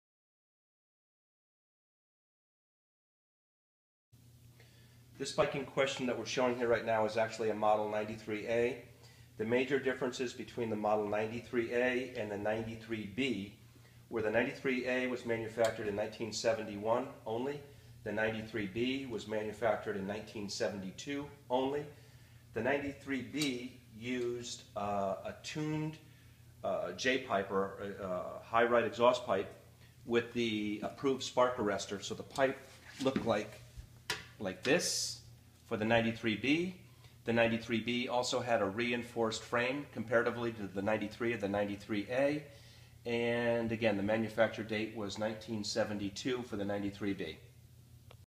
Hodaka (93B) Super Rat Model Specifications
Paul Stannard, owner of Strictly Hodaka Motorcycles, explains the characteristics of a (93B) Super Rat. See schematics and more specifications on Strictly Hodaka's